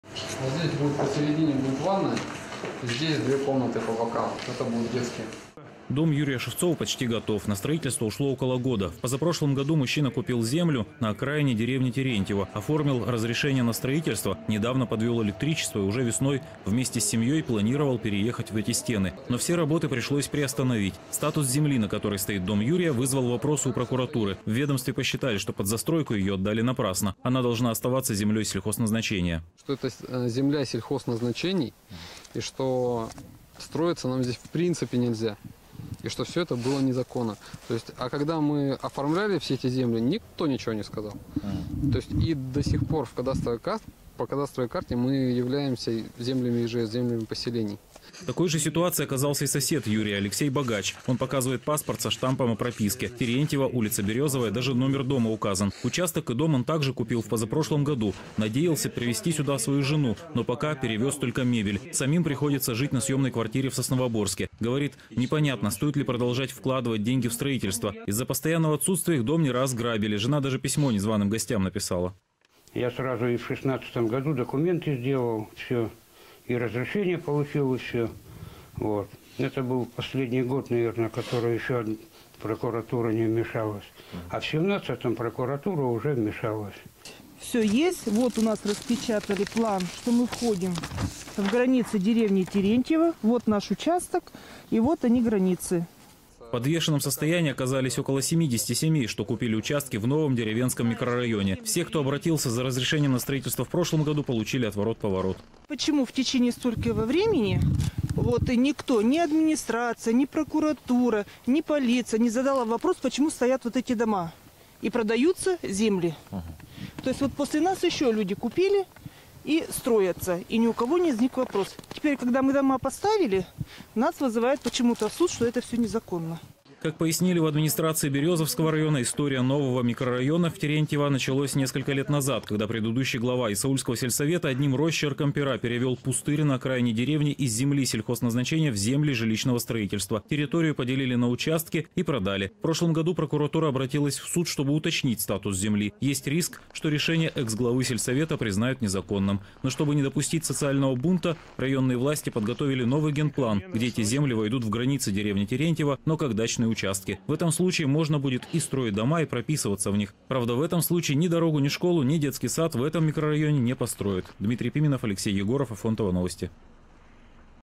Почти 70 семей в деревне Терентьево могут лишиться своих земельных участков
Почти 70 семей в деревне Терентьево могут лишиться своих земельных участков. Люди оформили их в собственность, многие уже построили дома и планировали скорый переезд. Но прокуратура посчитала, что земля была переведена под застройку неправильно и через суд требует вернуть ей статус земли сельхозназначения.